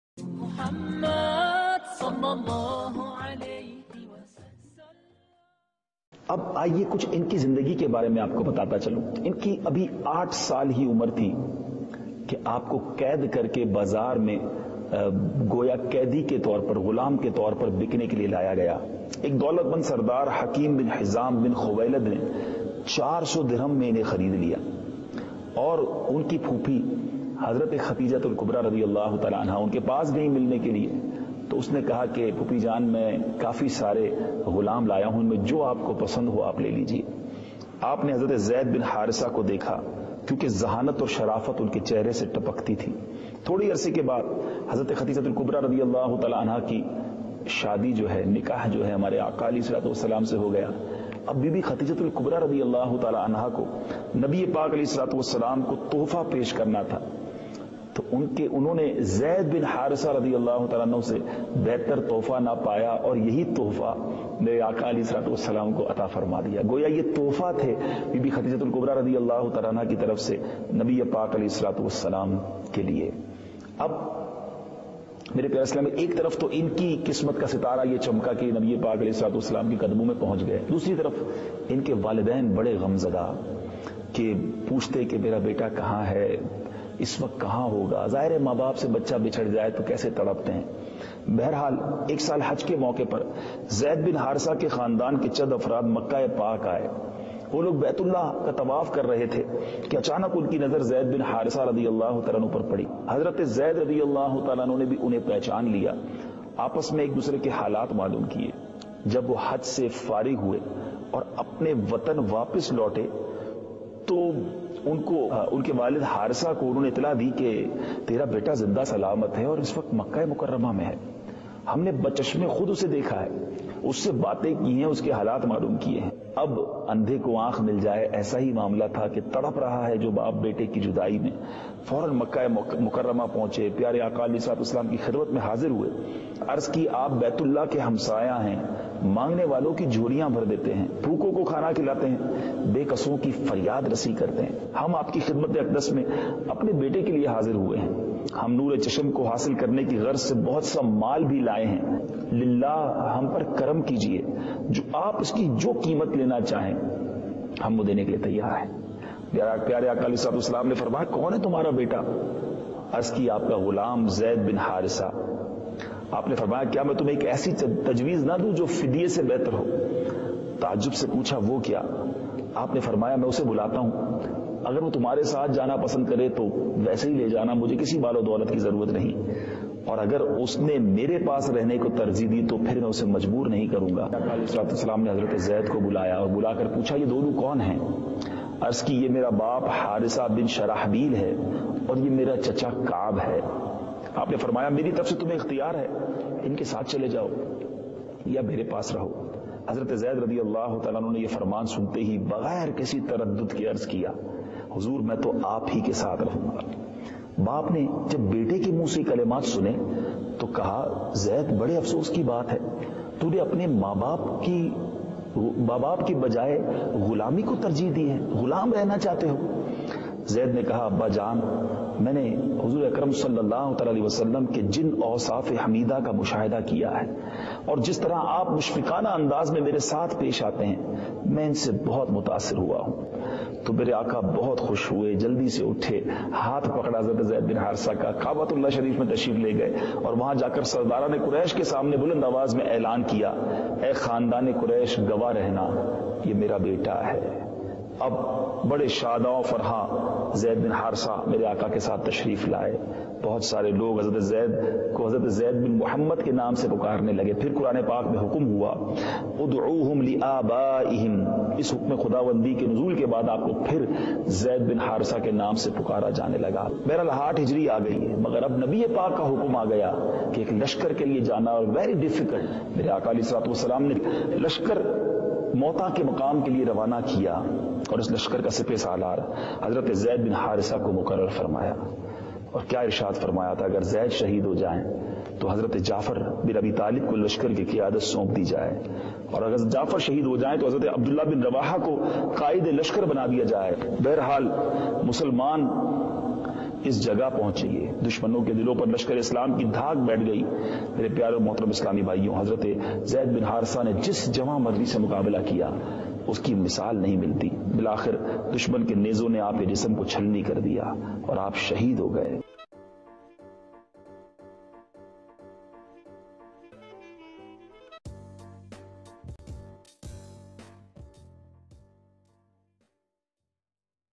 THE COMPANION: Hazrat Zaid Bin Harish | Kerak, Jordan
Google Map Location :
Maqam Ja’far ibn Abi Talib
Maqam Zayd ibn Harithah
Kerak, Jordan

HAZRAT ZAID BIN HARITHA

Hazrat Zayd ibn Harithah
He was a companion of Muhammad ﷺ  who was at one stage regarded as his (adoptive) son. He is the only companion who is mentioned by name in the Qur'an  (33:37).

CAUSE OF DEATH : Martyred in the Battle of Mu'tah.

RESTING PLACE : al-Mazar, Mu'tah, Jordan

KNOWN FOR : being a Companion of the Prophet ﷺ.

ARABIC : زيد بن حارثة.

Assalaumalaikum Friends,

JazakAllahKhair.